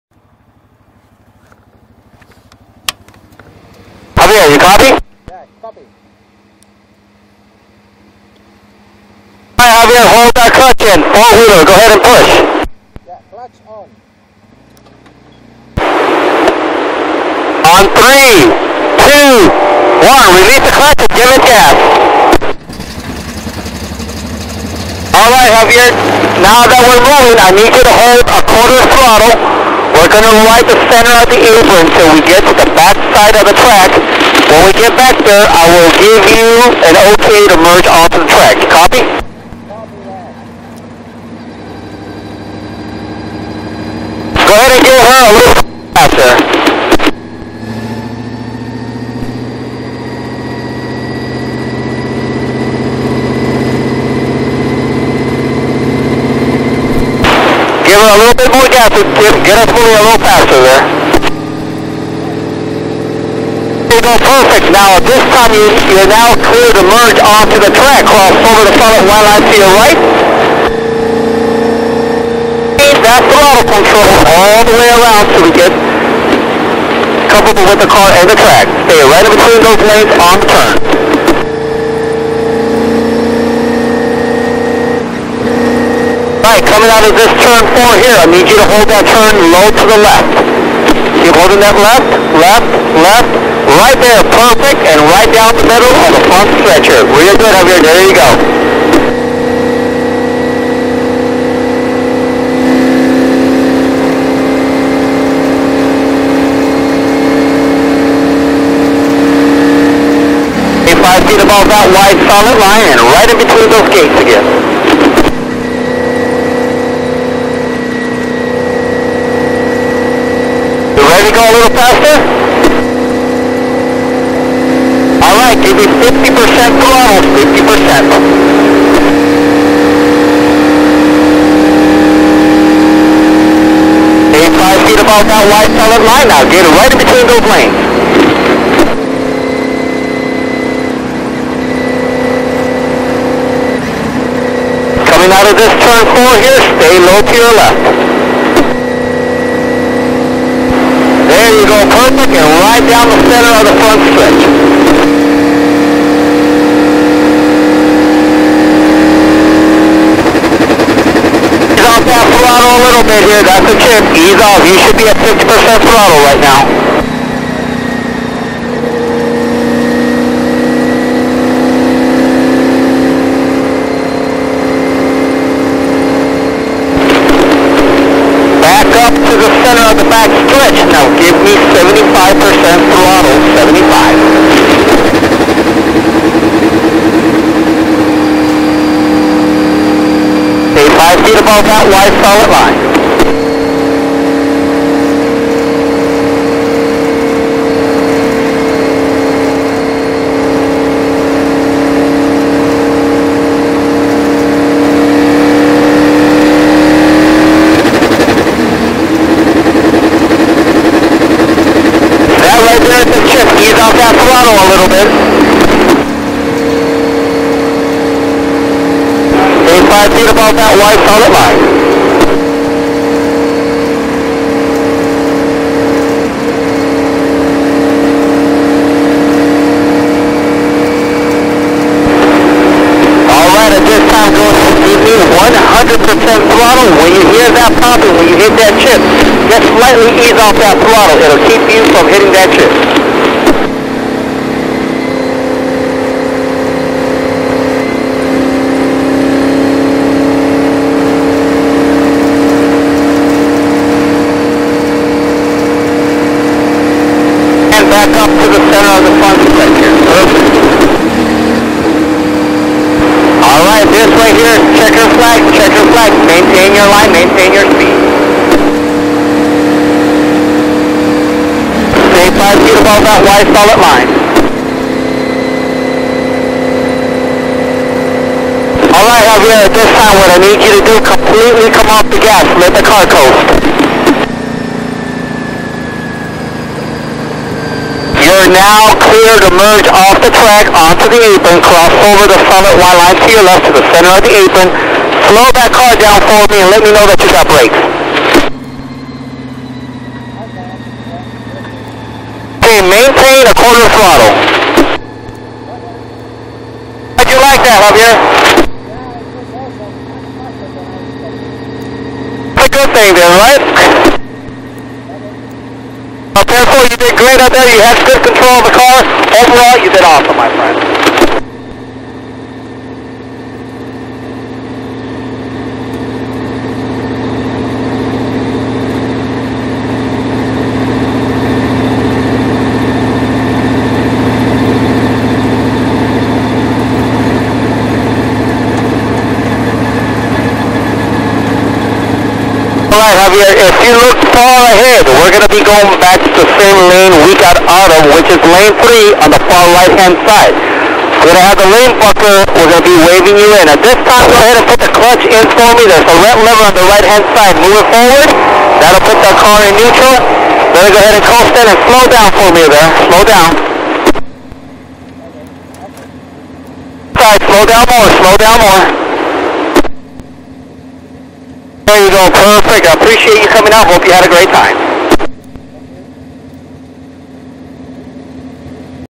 Let's Drive # 22 Indylights at Las Vegas Motor Speedway
Just a quickrun on a single-seater. Top Speed was 151 mph. Quite an experience.